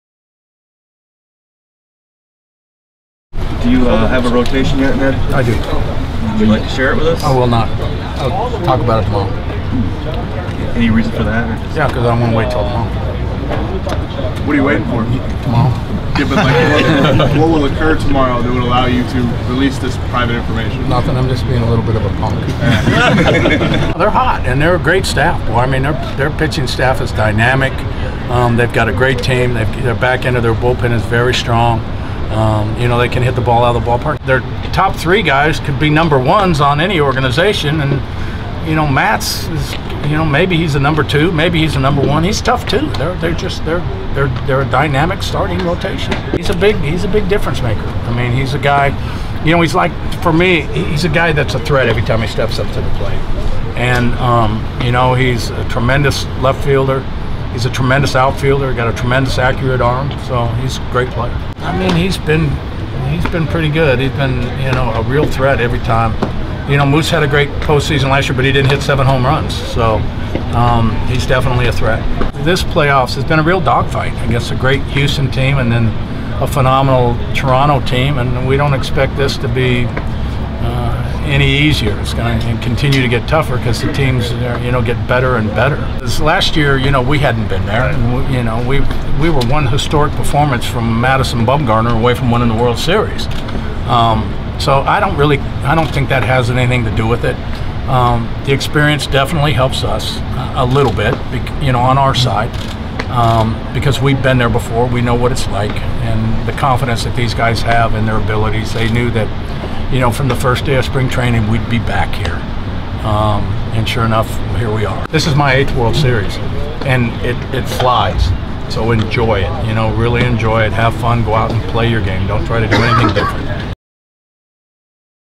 Ned Yost: "We don't expect this to be any easier"
Royals manager Ned Yost previewed the upcoming World Series against the Mets on Sunday before his team worked out at Kauffman Stadium. Game 1 is 7 p.m. Tuesday at The K (TV: Fox).